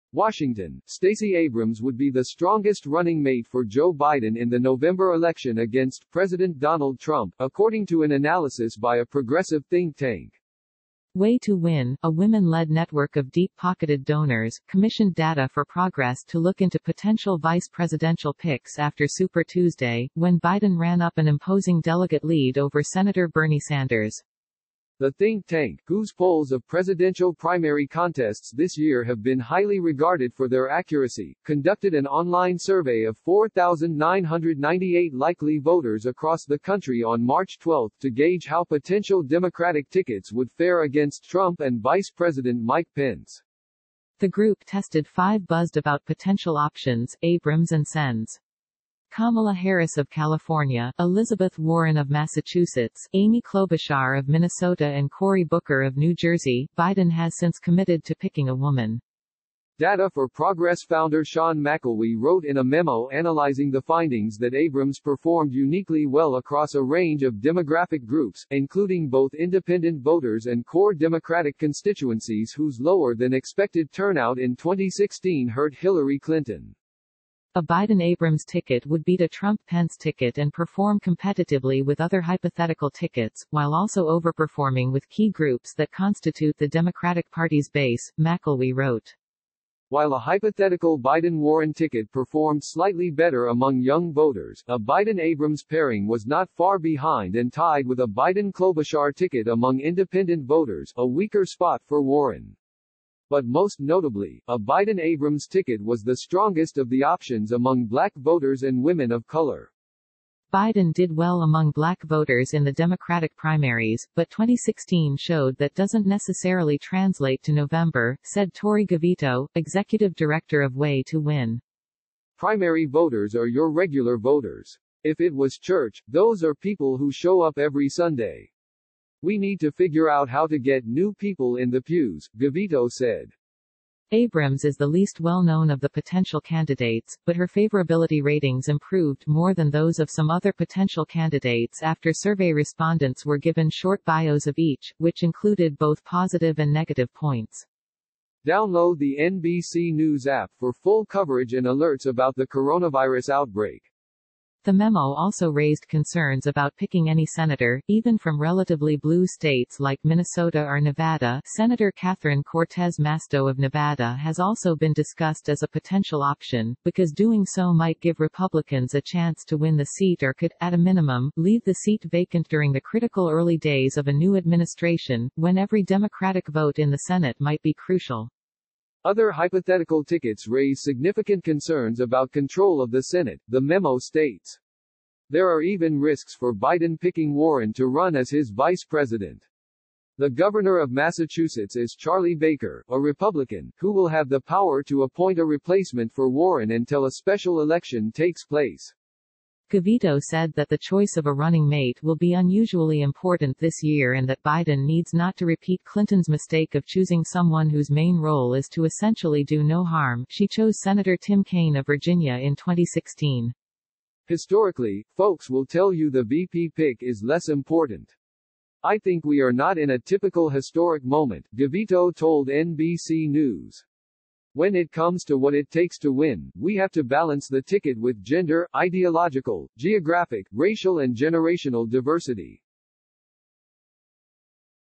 Progressive group's analysis finds Stacey Abrams is Biden's best choice. Here's why.
WASHINGTON —  Stacey Abrams  would be the strongest  running mate  for Joe Biden in the November election against President Donald Trump, according to an analysis by a progressive think tank.